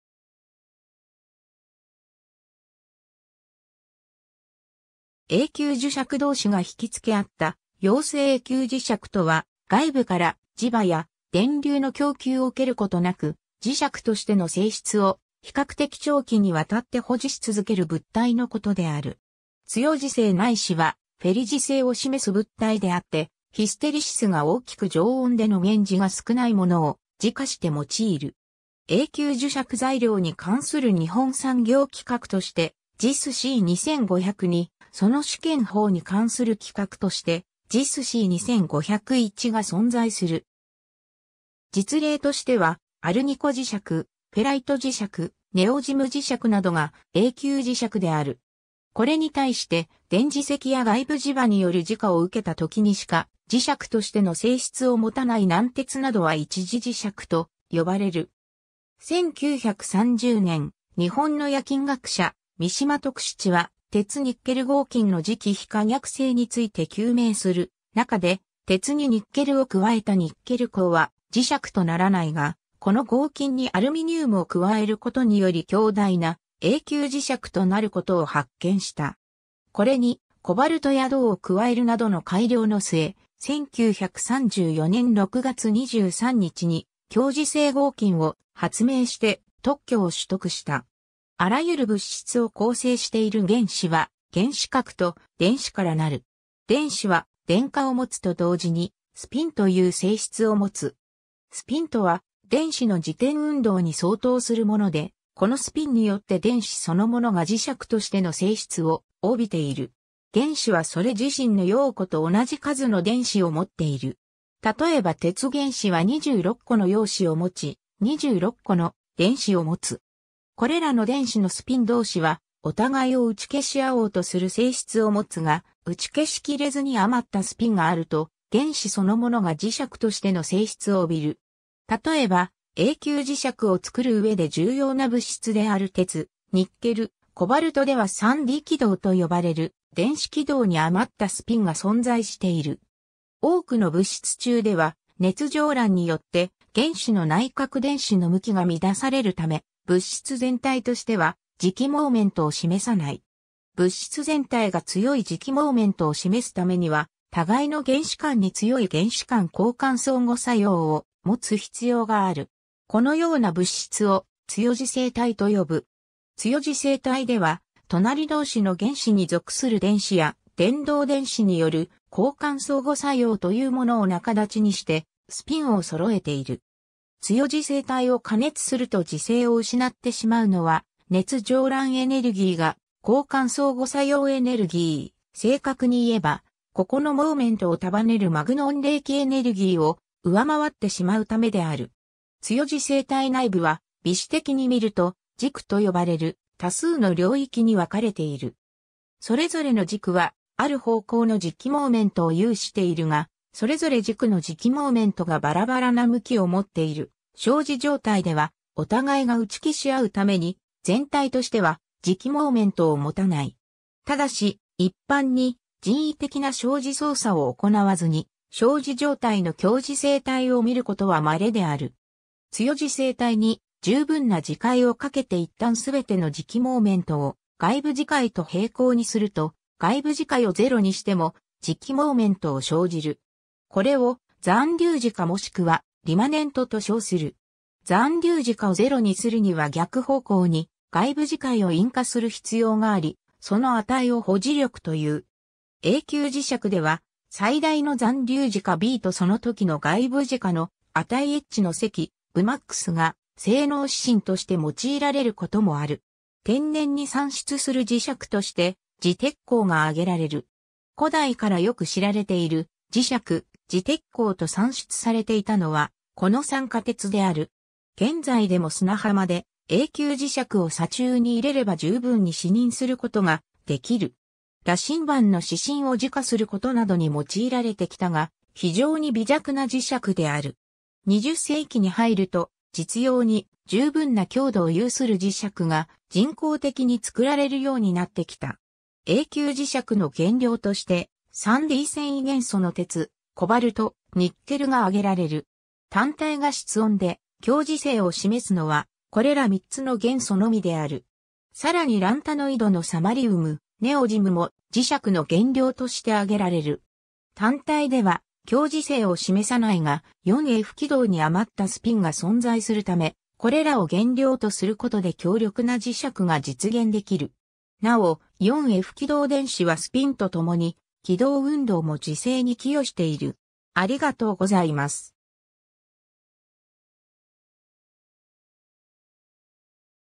永久磁石
永久磁石どうしが引きつけ合った様子 永久磁石（えいきゅうじしゃく、permanent magnet）とは、外部から磁場や電流の供給を受けることなく磁石としての性質を比較的長期にわたって保持し続ける物体のことである。
強磁性ないしはフェリ磁性を示す物体であってヒステリシスが大きく常温での減磁が少ないものを磁化して用いる。
永久磁石材料に関する日本産業規格としてJIS C 2502、その試験法に関する規格としてJIS C 2501が存在する。
 実例としてはアルニコ磁石、フェライト磁石、ネオジム磁石などが永久磁石である。
これに対して、電磁石や外部磁場による磁化を受けた時にしか磁石としての性質を持たない軟鉄などは一時磁石と呼ばれる。
 1930年（昭和5年）、日本の冶金学者三島徳七は、鉄‐ニッケル合金の磁気非可逆性について究明する中で、鉄にニッケルを加えたニッケル鋼は磁石とならないが、この合金にアルミニウムを加えることにより強大な永久磁石となることを発見した。
これにコバルトや銅を加えるなどの改良の末、1934年（昭和9年）6月23日に、強磁性合金を発明して特許を取得した（96371号）。
 あらゆる物質を構成している原子は原子核と電子からなる。
電子は電荷を持つと同時に、スピンという性質をもつ。
 スピンとは電子の自転運動に相当するもので、このスピンによって電子そのものが磁石としての性質を帯びている。
 原子はそれ自身の陽子と同じ数の電子を持っている。
例えば鉄原子は26個の陽子を持ち、26個の電子を持つ。
これらの電子のスピン同士はお互いを打ち消しあおうとする性質を持つが（フントの規則）、打ち消しきれずに余ったスピンがあると、原子そのものが磁石としての性質を帯びる。
例えば、永久磁石を作る上で重要な物質である鉄、ニッケル、コバルトでは3d軌道と呼ばれる電子軌道に余ったスピンが存在している。
 多くの物質中では熱擾乱によって原子の内殻電子の向きが乱されるため、物質全体としては磁気モーメントを示さない。
物質全体が強い磁気モーメントを示すためには、互いの原子間に強い原子間交換相互作用を持つ必要がある。
このような物質を強磁性体と呼ぶ。
強磁性体では隣同士の原子に属する電子や伝導電子による「交換相互作用」というものを仲立ちにしてスピンをそろえている。
強磁性体を加熱すると磁性を失ってしまうのは、熱擾乱エネルギーが交換相互作用エネルギー、正確に言えばここのモーメントを束ねるマグノン励起エネルギーを上回ってしまうためである。
 強磁性体内部は微視的に見ると「磁区」とよばれる多数の領域に分かれている。
それぞれの磁区はある方向の磁気モーメントを有しているが、それぞれ磁区の磁気モーメントがばらばらな向きを持っている消磁状態では、お互いが打ち消しあうために、全体としては磁気モーメントを持たない。
ただし、一般に人為的な消磁操作を行わずに消磁状態の強磁性体を見ることは稀である。
 強磁性体に十分な磁界をかけて一旦すべての磁気モーメントを外部磁界と平行にすると、外部磁界をゼロにしても磁気モーメントを生じる。
これを残留磁化もしくはリマネントと称する。
残留磁化をゼロにするには逆方向に外部磁界を印加する必要があり、その値を保磁力という。
永久磁石では最大の残留磁化Bとそのときの外部磁化の値Hの積BHmaxが性能指針として用いられることもある。
 天然に産出する磁石として磁鉄鉱(Fe3O4)（マグネタイト）が挙げられる。
古代からよく知られている磁石、磁鉄鉱（乃至は砂鉄）と産出されていたのはこの酸化鉄である。
現在でも砂浜で永久磁石を砂中にいれれば十分に視認することが出来る。
羅針盤の指針を磁化することなどに用いられてきたが、非常に微弱な磁石である。
20世紀に入ると、実用に十分な強度を有する磁石が人工的に作られるようになってきた。
 永久磁石の原料として、3d遷移元素の鉄、コバルト、ニッケルが挙げられる。
単体が室温で強磁性を示すのは、これら3つの元素のみである。
さらにランタノイドのサマリウム、ネオジムも磁石の原料として挙げられる。
単体では強磁性を示さないが、4f軌道に余ったスピンが存在するため、これらを原料とすることで強力な磁石が実現できる。
なお、4f軌道電子はスピンと共に軌道運動も磁性に寄与している。